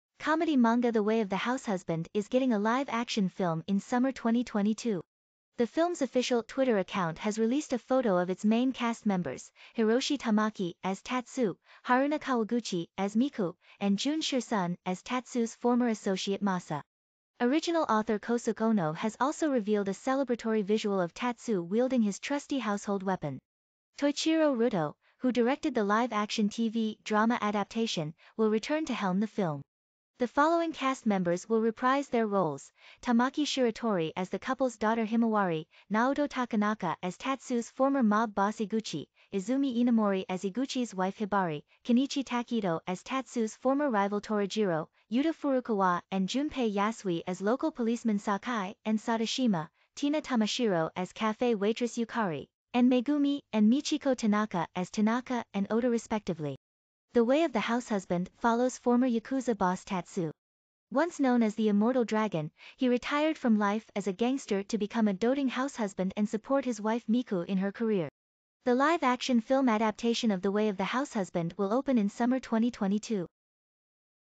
The way of Housewives makes live action movies!
LIFE TIPS he retired from life as a gangster to become a doting househusband and support his wife Miku in her career. The live action film adaptation of  The Way of the Househusband will open in summer 2022. Comedy manga  The Way of the Househusband is getting a live action film in summer 2022 Naoto Takenaka as Tatsu’s former mob boss Eguchi and Megumi and Michiko Tanaka as Tanaka and Ota respectively. The Way of the Househusband follows former yakuza boss Tatsu. Once known as the Hiroshi Tamaki as Tatsu The film's official Twitter account has released a photo of its main cast members